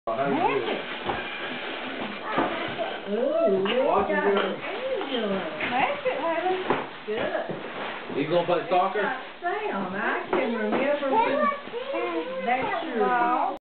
Hayley & her first remote car
Opening gifts the Saturday before Christmas at Nannies, December 18, 2010.